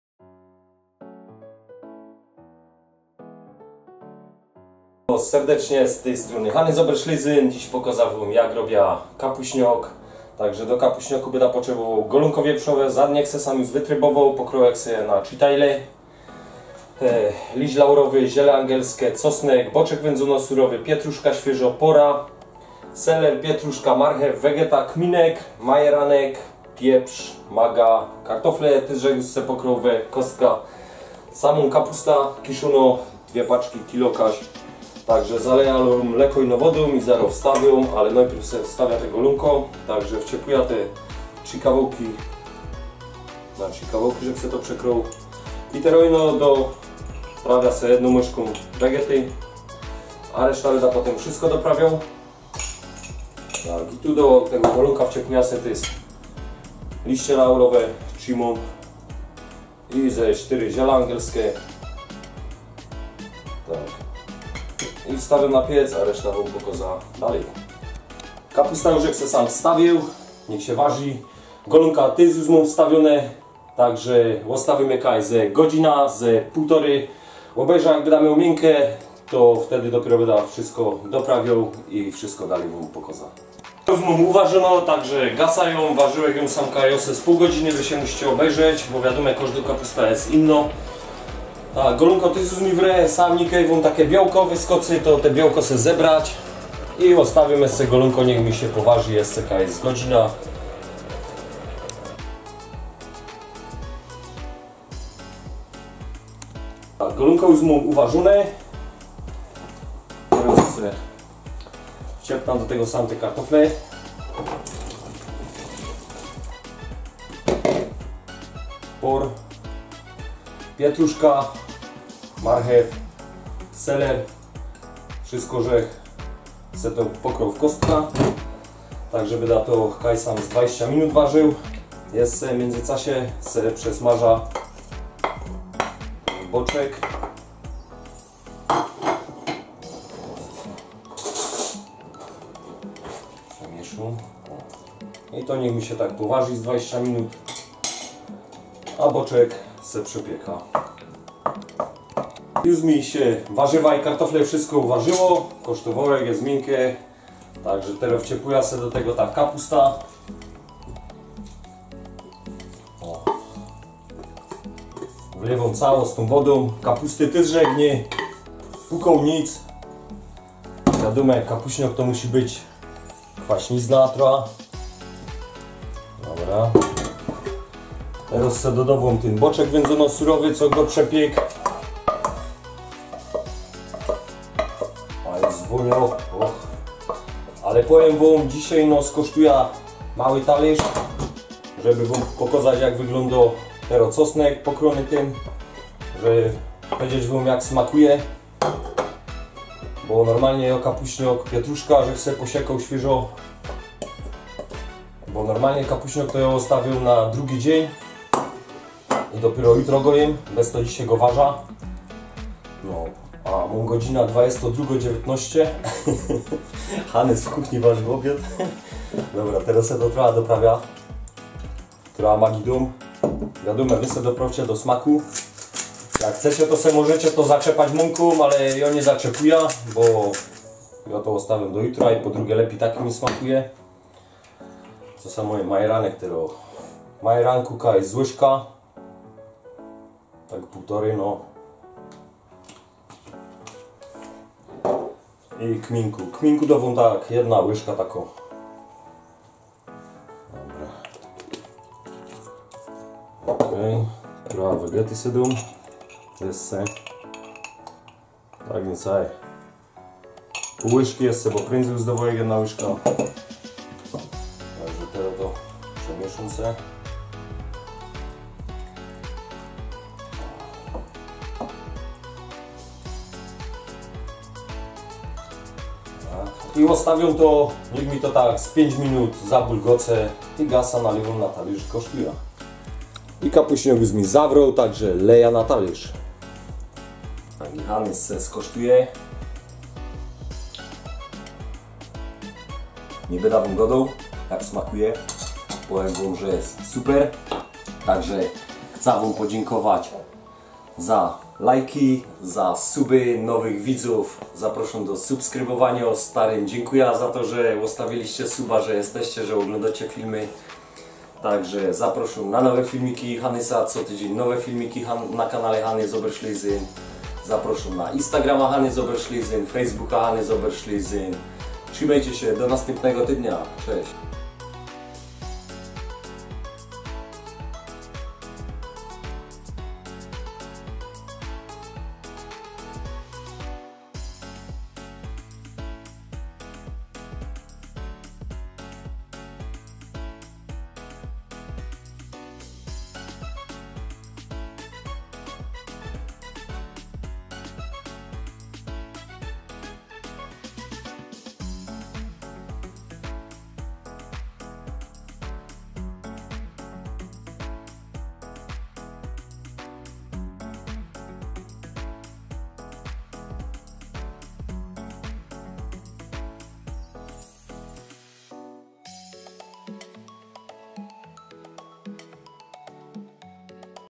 Kapuśniak z Kiszonej Kapusty na Golonku
Witom Serdecznie.🤗😊

Składniki: 😊👇👇👇😊

Kapusta Kiszona 1kg
Woda 3l
Golonko 1szt
3 Liście Laurowe
Ziele Angielskie 4szt
Czosnek 3 Ząbki
Vegeta 1,5 Łyżki
Kminek 1 Łyżka
Majeranek 1,5 Łyżki
Kartofle 10szt Pokroić w Kostka
Pora 0,5szt
Seler 200g
Pietruszka 150g
Marchew 250g
Boczek 250g
Doprawić Maga Pieprz Do smaku Lepiej Smakuje na 2 Dzień

Zapraszam na: 😊👇👇😊🌏💻

Smacznego Życzy Hanys 😊🍜😊